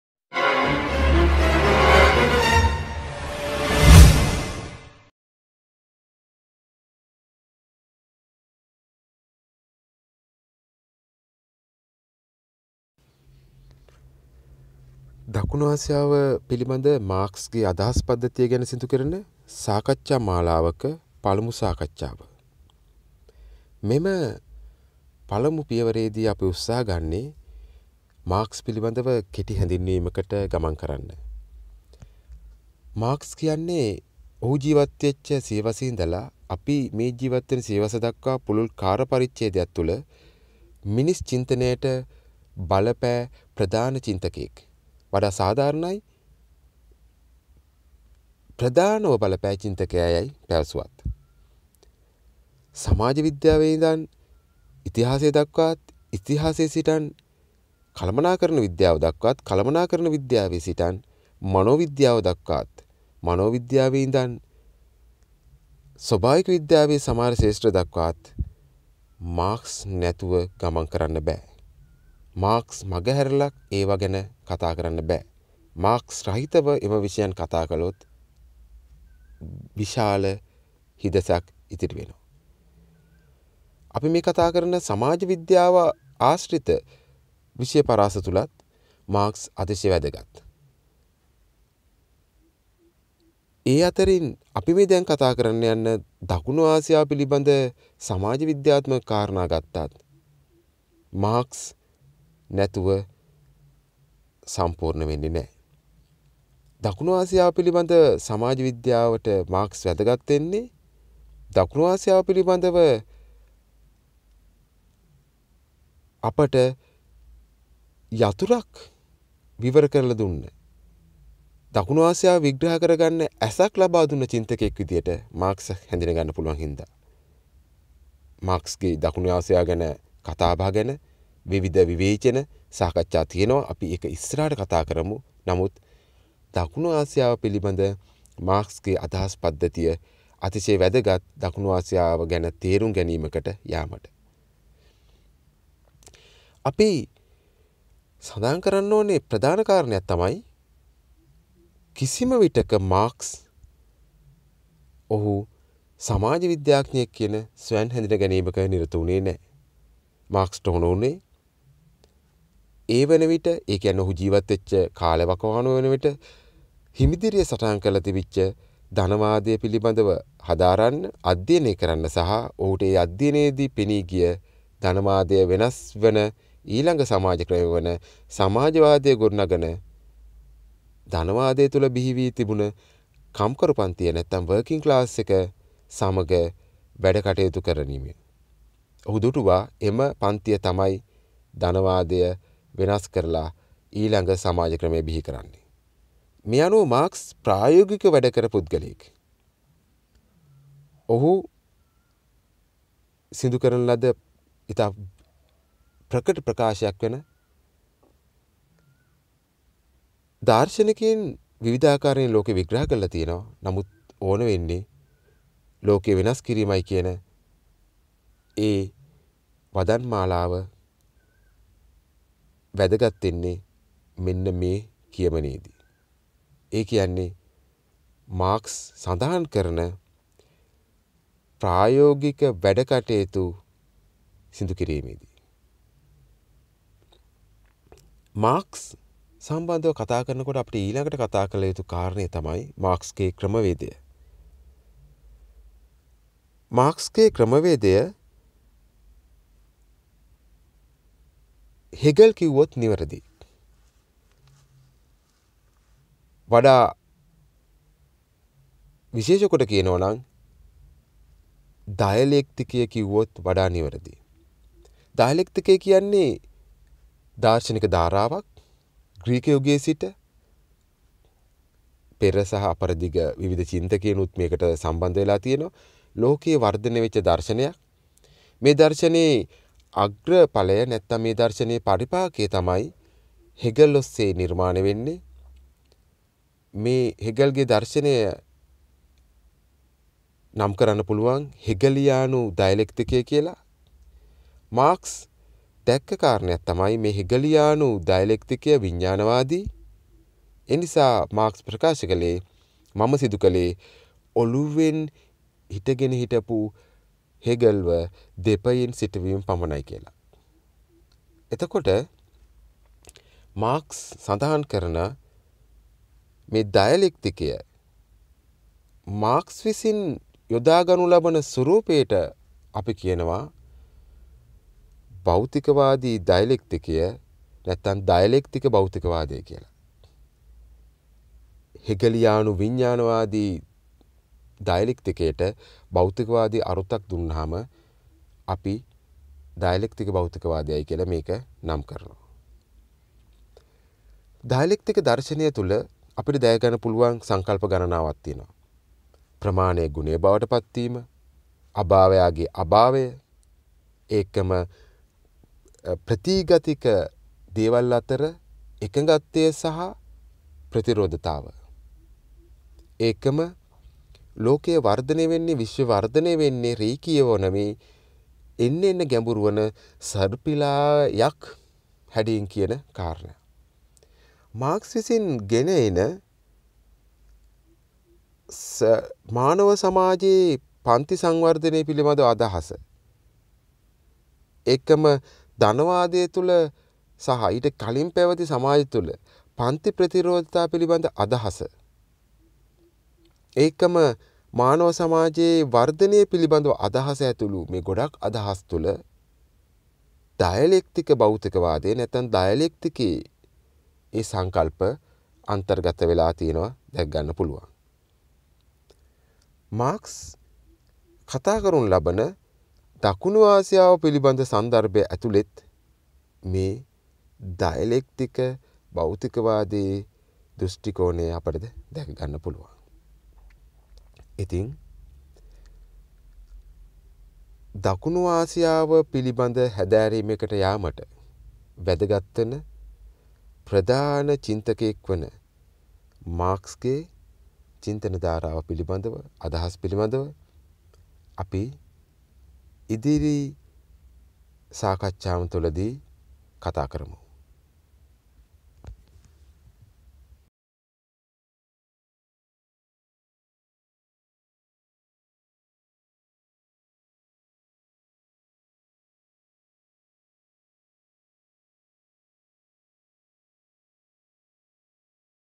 දකුණු ආසියාව ගැන මාක්ස් -01
දකුණු ආසියාව පිළිබඳ මාක්ස්ගේ අදහස් පද්ධතිය ගැන ප්‍රභාත් ගලගමගේ සිදුකරන සාකච්ඡා මාලාවක පළමු සාකච්ඡාව